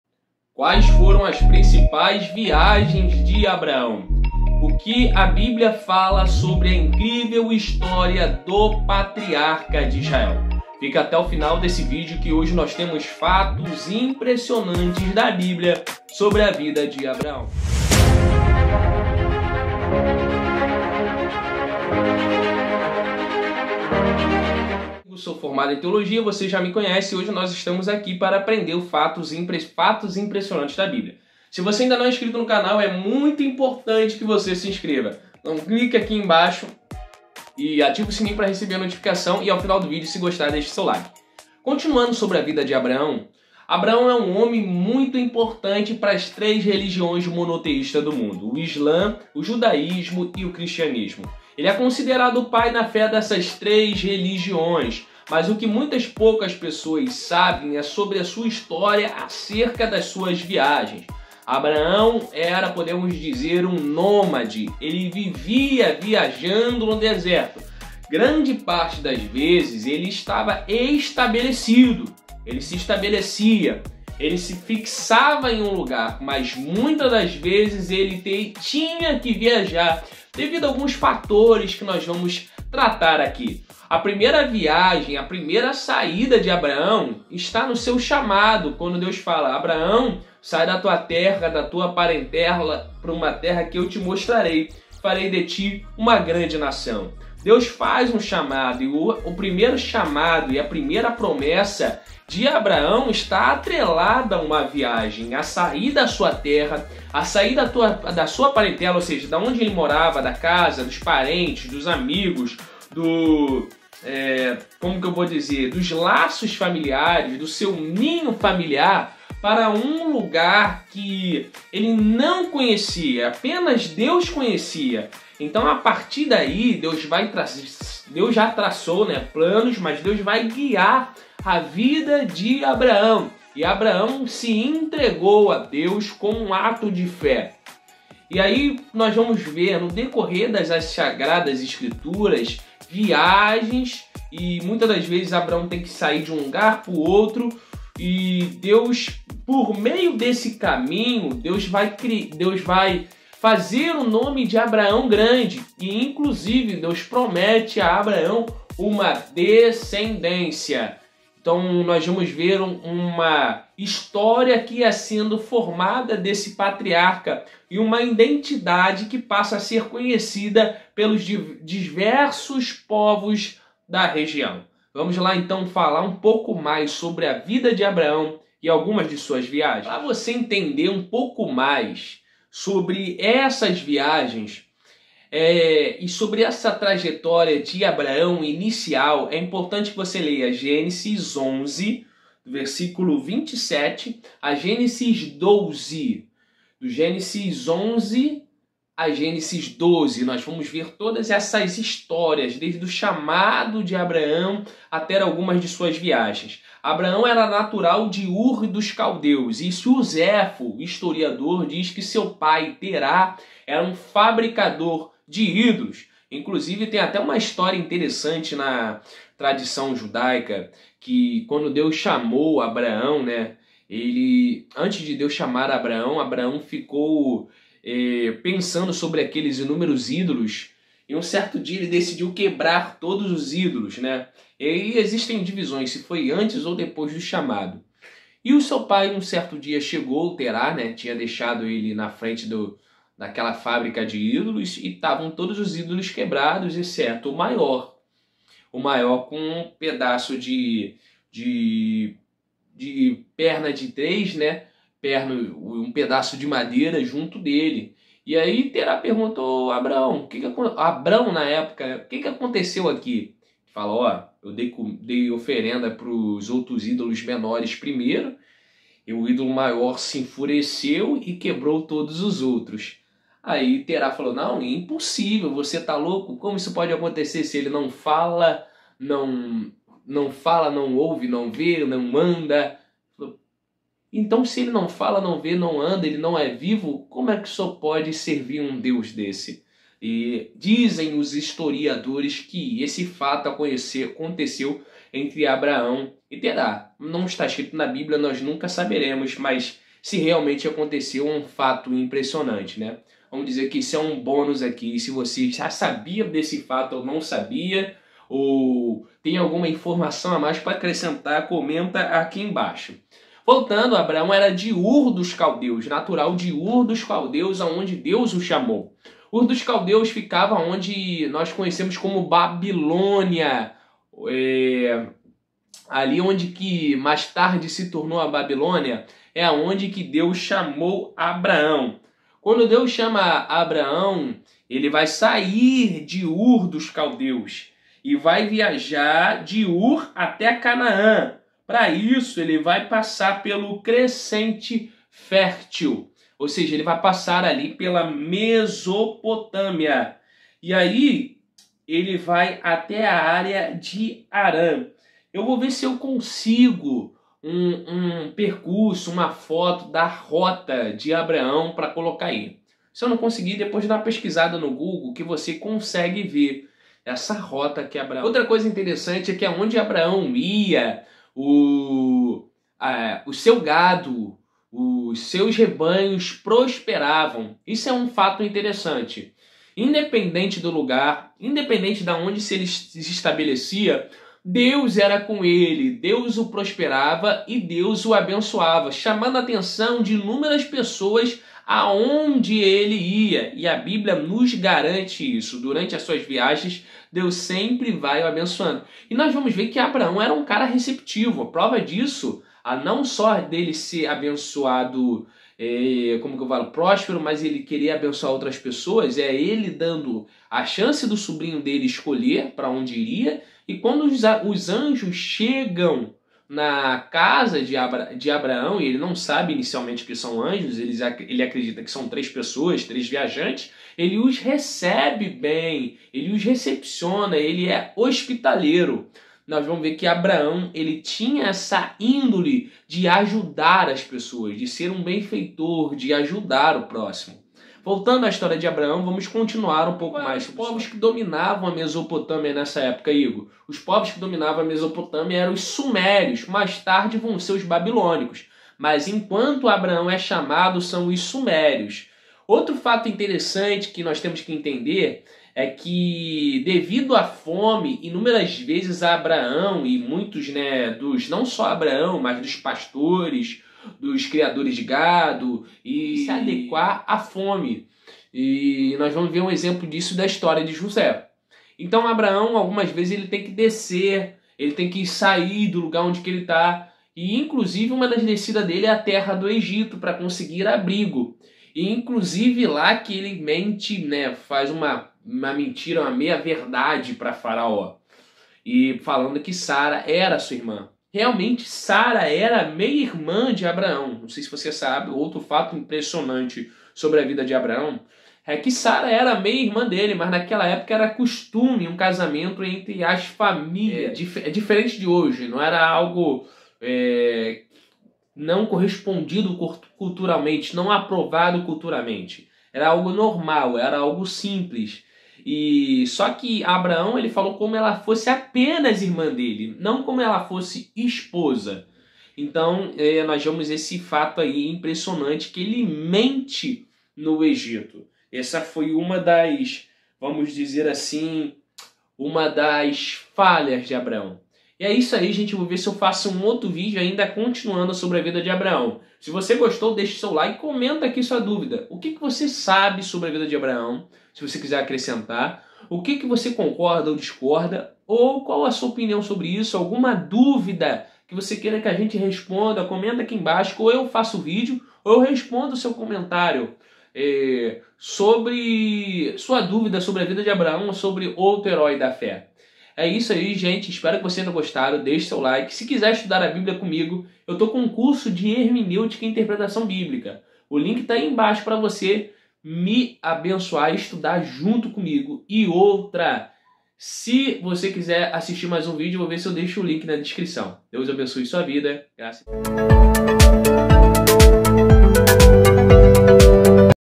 A peregrinação de Abraão começou em Ur dos Caldeus? | Descubea porque Abraão saiu de Canaã
Qual foi o percurso de Abraão? Conheça a rota de Ur  (cujas ruínas estão hoje no Iraque) até Canaã que Abraão fez com que muitas famílias parassem ao longo da difícil jornada